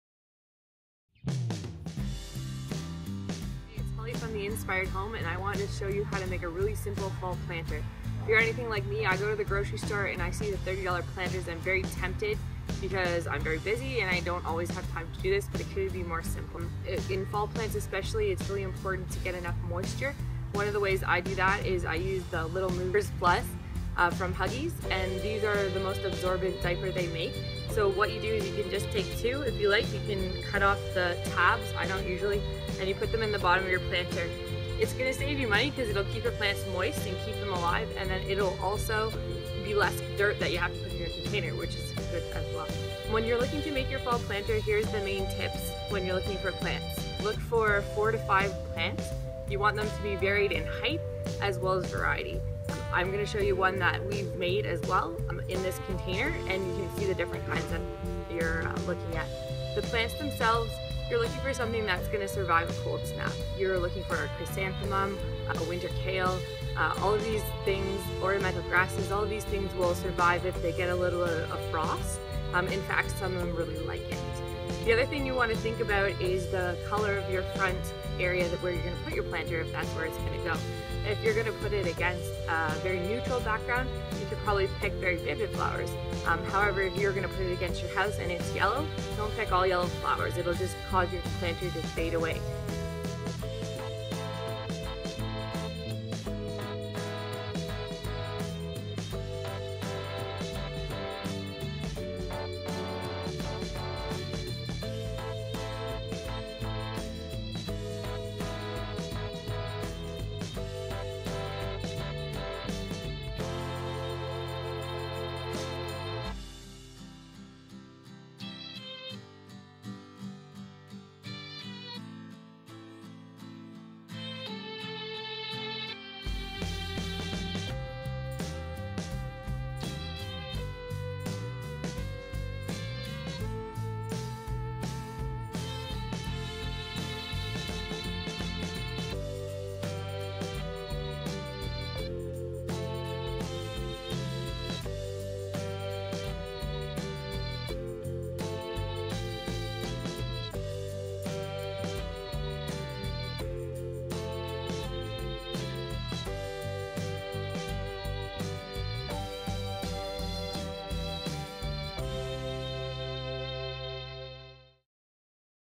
DIY Fall Planters Made Simple
DIY Fall Planters Made Simple using Little Movers Plus, a variety of cold hardy plants & more.